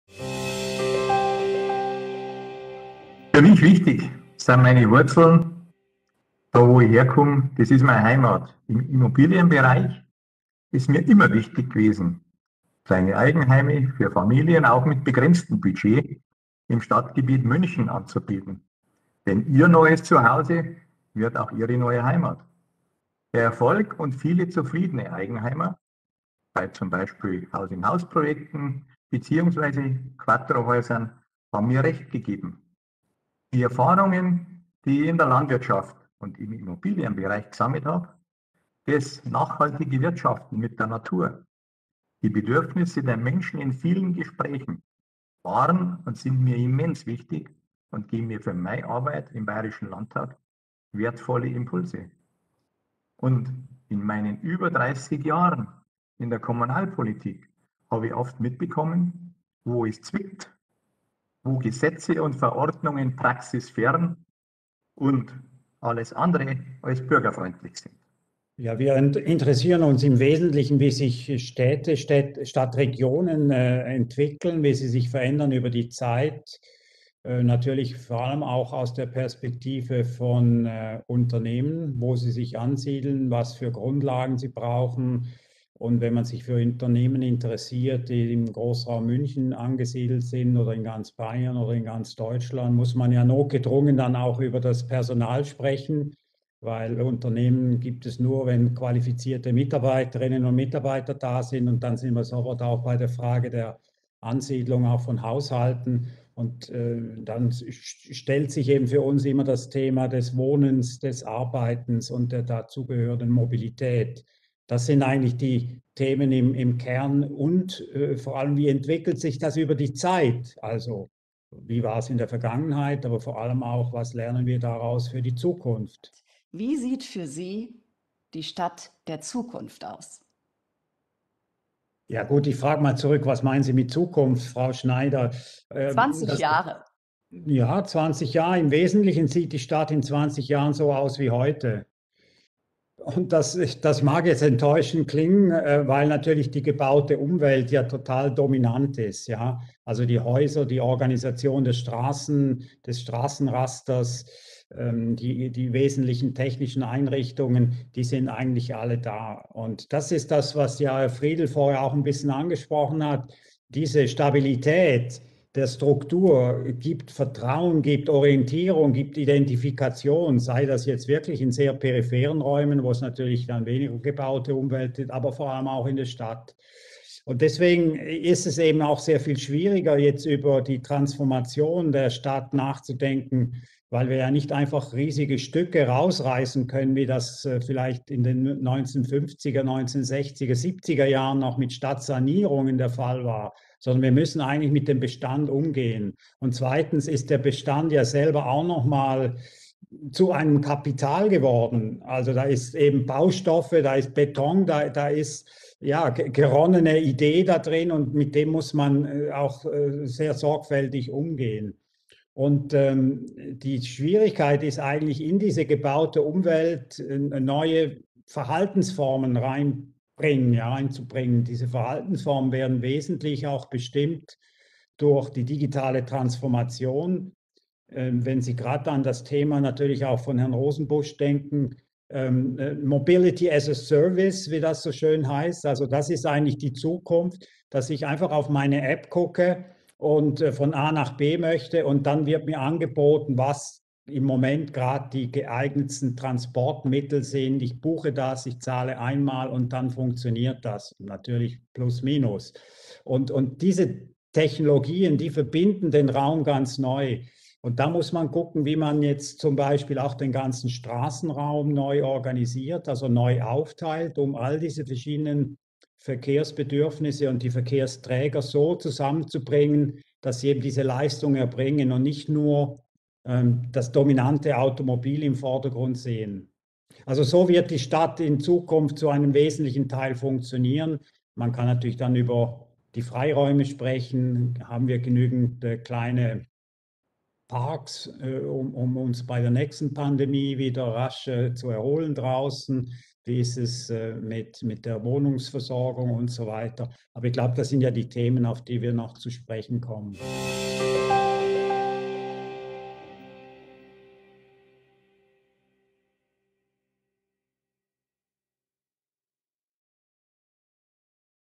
Blickpunkt B@yern: Unsere Gäste stellen sich vor
Was sind die Aufgaben des Instituts für Raumentwicklung an der TU München? Vor welchen Herausforderungen stehen die Münchner Verkehrsbetriebe? Und welche Erfahrungen machen Hans Friedl, Sprecher für Wohnen und Bauen der FREIE WÄHLER-Fraktion, zum Immobilienexperten? Das verrieten Prof. Dr. Alain Thierstein, Professor für Raumentwicklung an der TU München, Dr. Bernd Rosenbusch, Geschäftsführer der Münchner Verkehrs- und Tarifverbund GmbH (MVV) und Hans Friedl der Fernsehmoderatorin Sylvia Schneider beim Online-Event "Blickpunkt B@yern: Wie wohnen wir in Zukunft?".